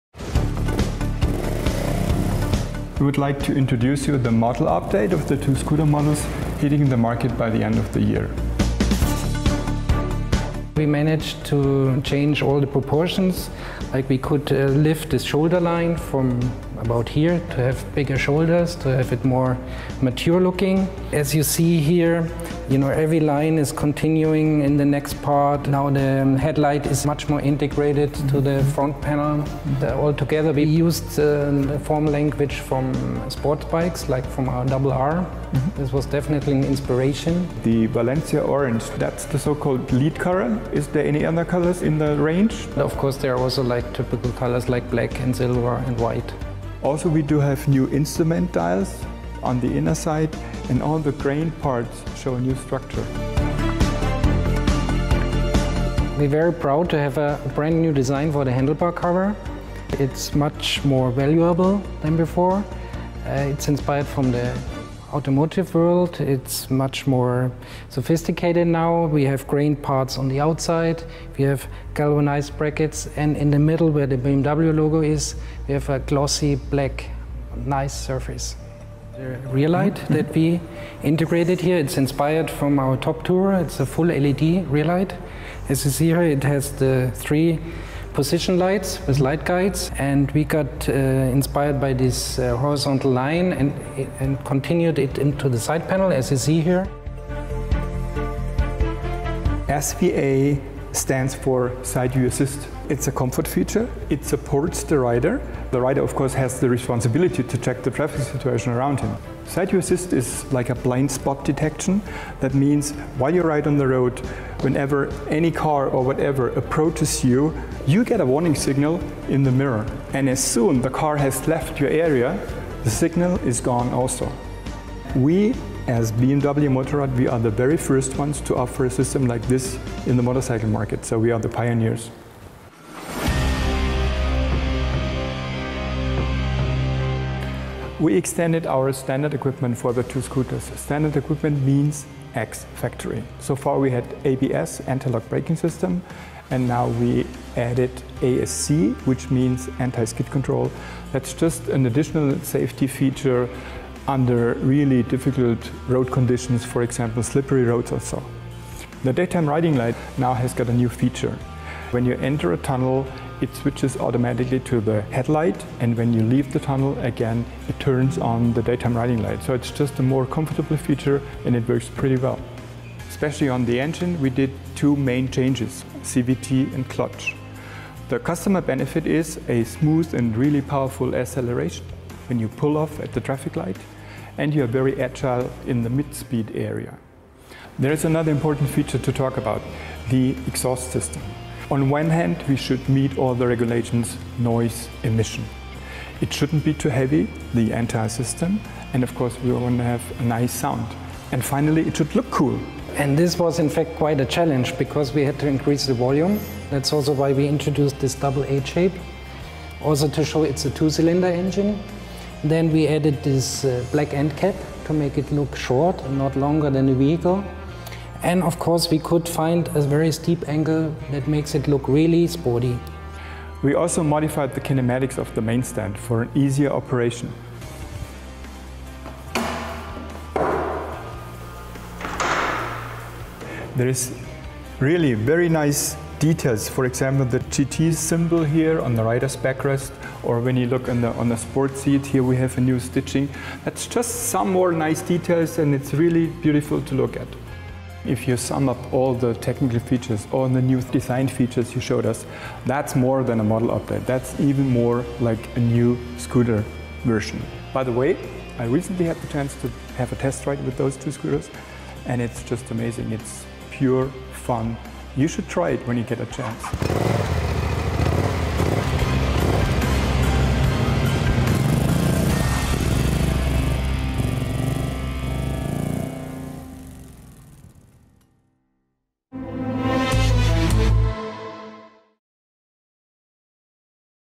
Nouveaux Scooter BMW C 650 Sport et C 650 GT
Découvrez dans le détail les nombreuses nouveautés et évolutions des nouveaux scooters BMW C 650 Sport et C 650 GT.